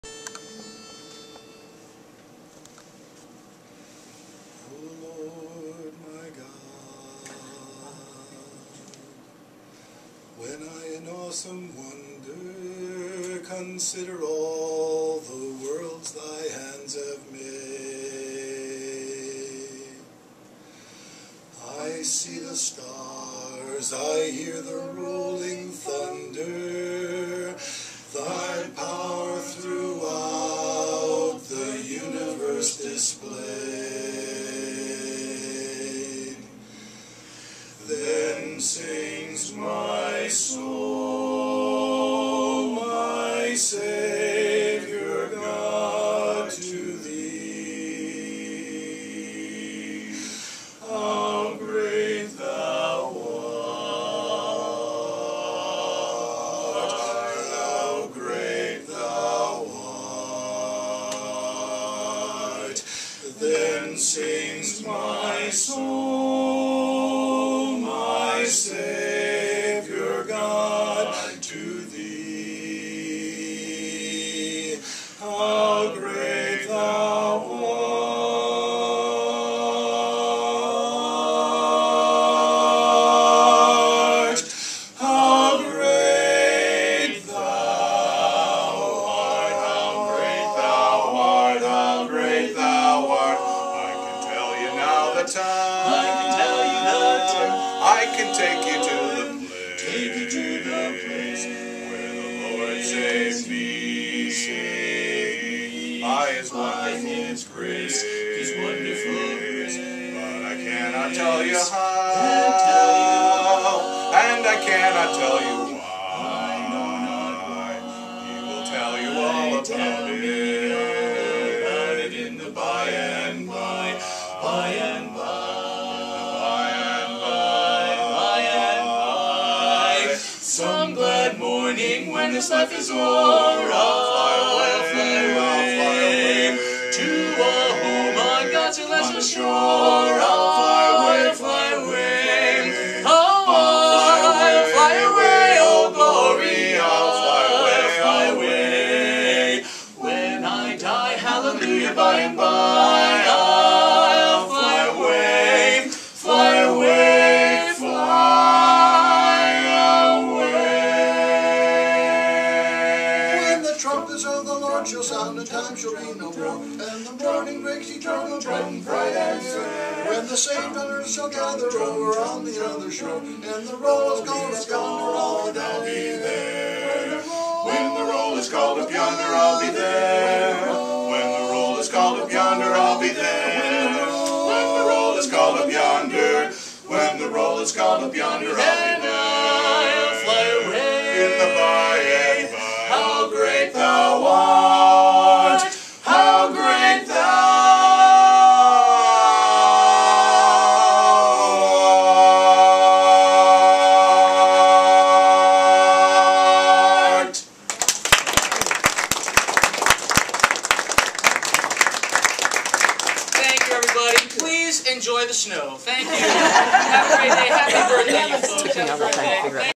Gospel Medley by Rat Race barbershop quartet
Rat Race 3.0 recently performed in Lebanon, NH